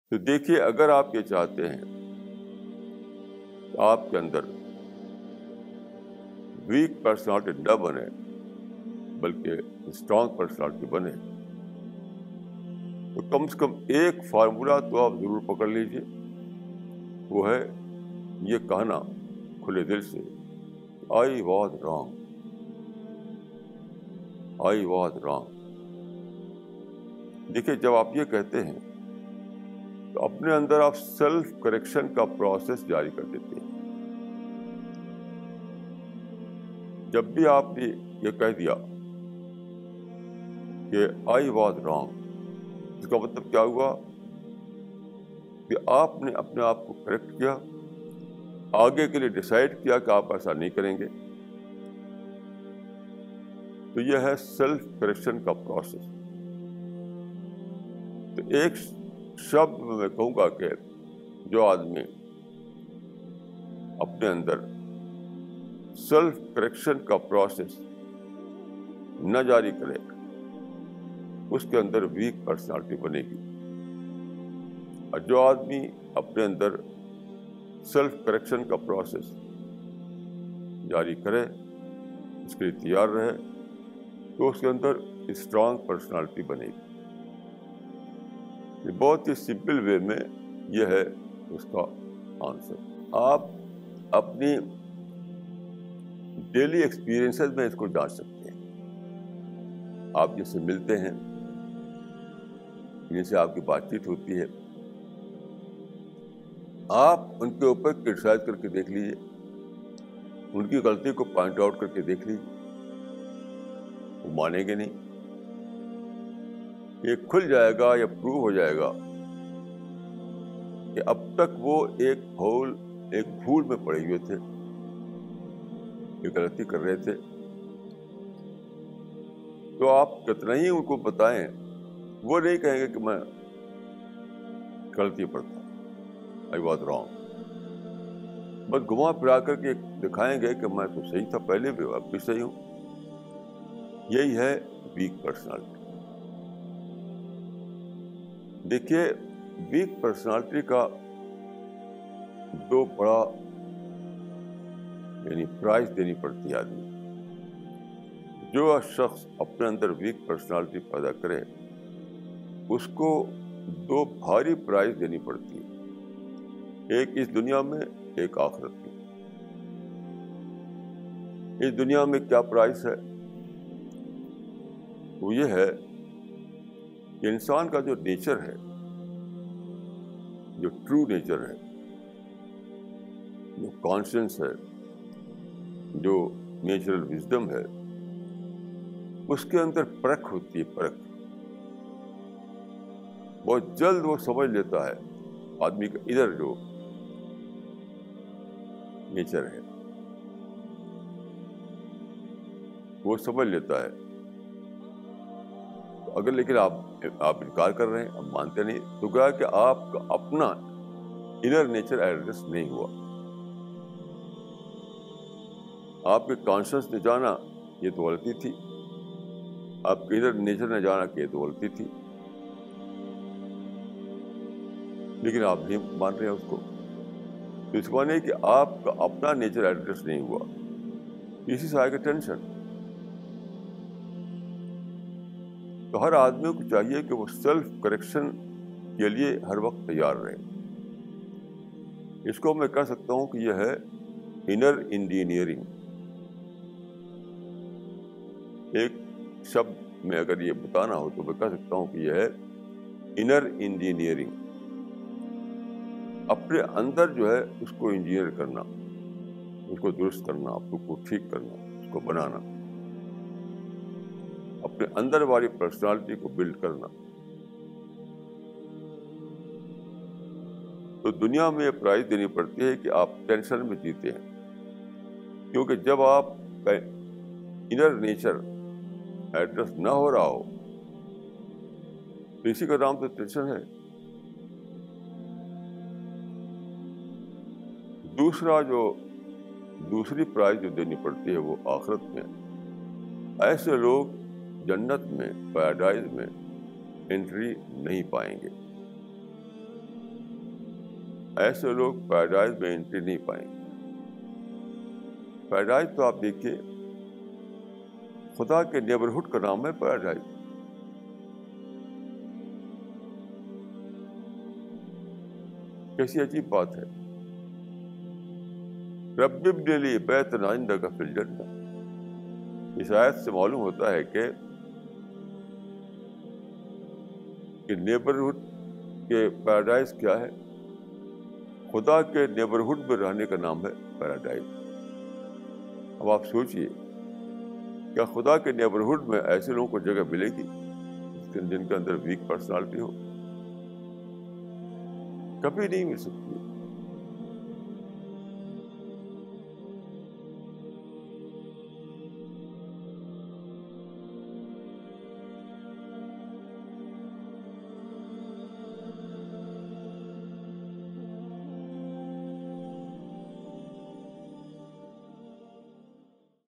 Don't Develop a Weak Personality
To become a strong personality one has to adopt the formula of openly saying "I was wrong". This develops a self correction process and makes a person strong personality. This inner engineering saves you from all kinds of stress and tension.